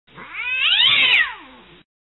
Cat Scream
Sound of an angry or afraid cat screaming. great for movie